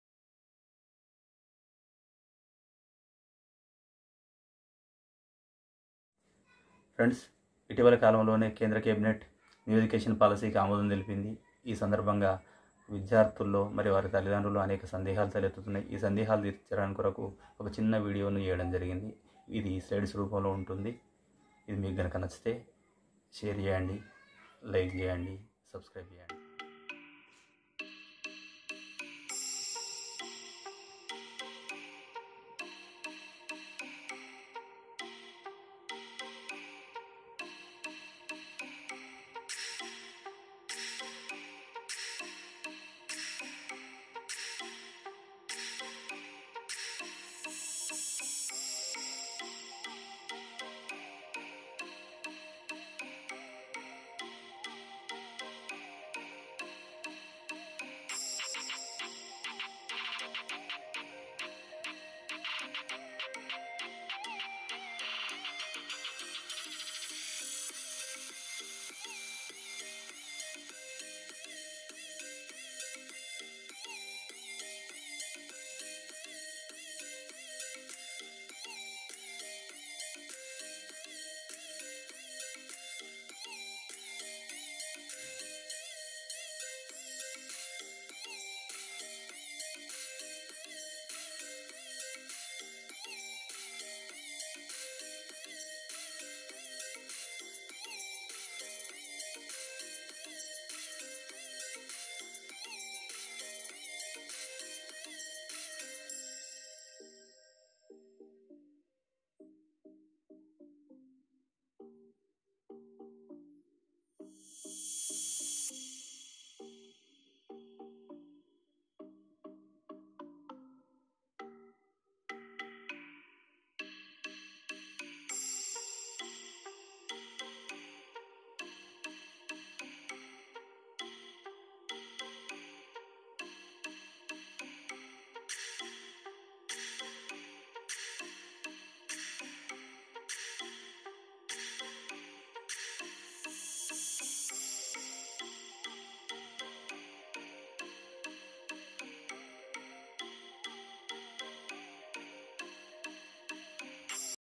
Indian Education policy 2020//major changes//in Telugu//slides..భారతీయ విద్యా వ్యవస్థ. భారీ మార్పులు
NEW EDUCATION POLICY-2020/How it affects on futue generation...It will bring changes in education structure,especially through improvement of skills  employment opportunities will be increased.students get more choice as they have multiple interests etc.// in Telugu//with the help of slides which useful to understand the major changes//vaishu creations